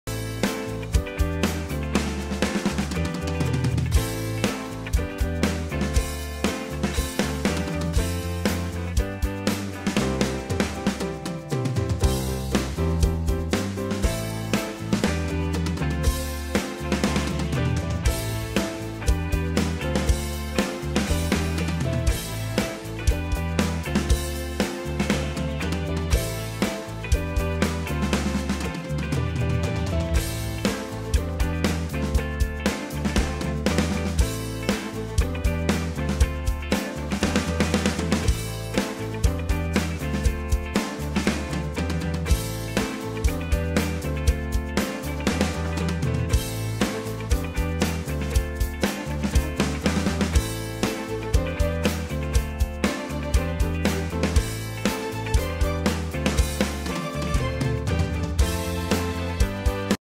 Drumming on the Roland TD-17 KVX
Drumming on the Roland TD-17 KVX TD17. Using Superior Drummer 3 VST with the Death and Darkness SDX Black  Drum kit. Mixed with Logic X Pro and UAD Arrow audio interface.